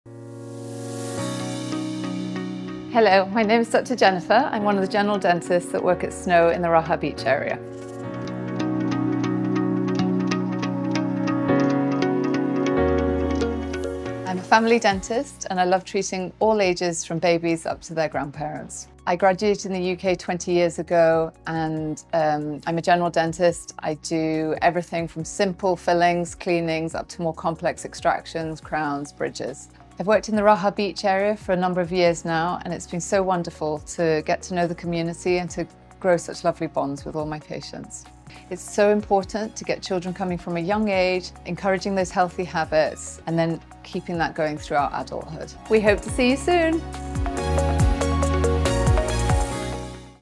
Meet Dr Jennifer your trusted family dentist at Snö Clinics.🌟
With 20 years of experience, she is dedicated to providing comprehensive dental care for patients of all ages, focusing on preventive and minimal invasive dentistry. ​

We can't wait to welcome you to our community of beautiful smiles!​ 😁✨

تعرفوا على الدكتورة جينيفر هوي، طبيبة أسنان في عيادات سنو لكل أفراد العائلة. 🌟
بخبرتها التي تتجاور عشرين عاما، تقدم علاجات شاملة في طب الأسنان لجميع الأعمار، وتتبع مبدأ الوقاية والحد الأدنى من التدخل العلاجي.

للمواعيد، تواصلوا معنا على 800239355 📞​
نرحب بكم في عالم الابتسامة الجميلة!​ 😁✨